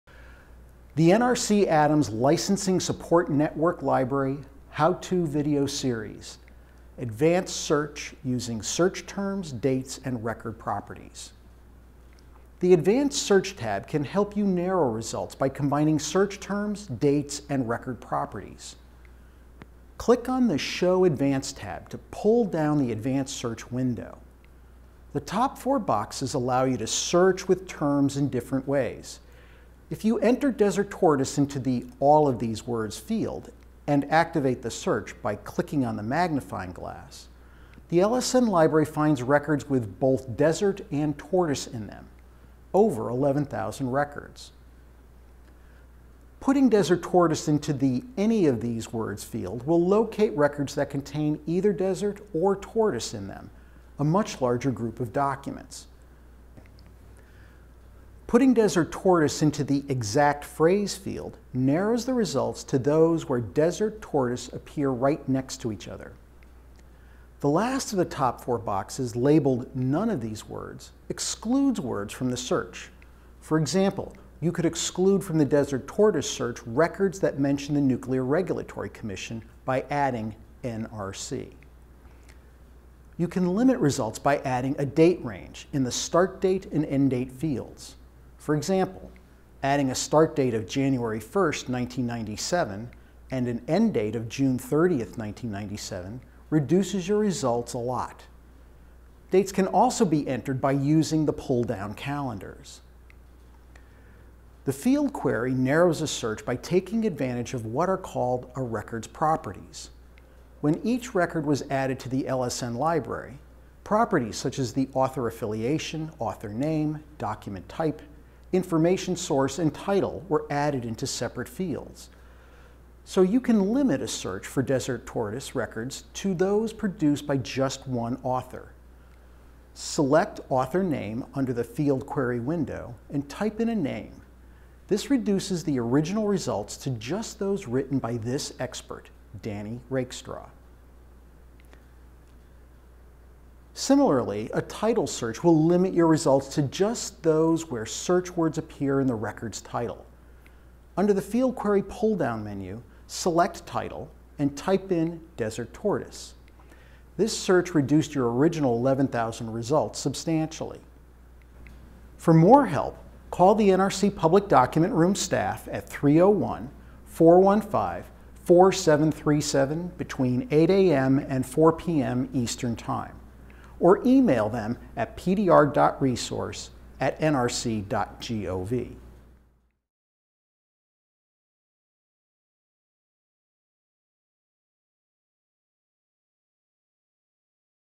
204: Advanced Search--Using Search Terms, Dates and Record Properties
NRC ADAMS LICENSING SUPPORT NETWORK LIBRARY HOW-TO VIDEO SERIES

This series of videos provides instruction for searching the NRC Licensing Support Network (LSN) library.

The LSN Library provides access to nearly 3.7 million publicly available documents related to the hearings regarding the Department of Energy’s application for authorization to construct a high-level nuclear waste geologic repository at Yucca Mountain, Nevada.

These short videos show how to access the LSN library and how to perform simple and advanced searches using accession numbers, keywords, date ranges, and other document properties.

You can access the LSN library through the NRC’s public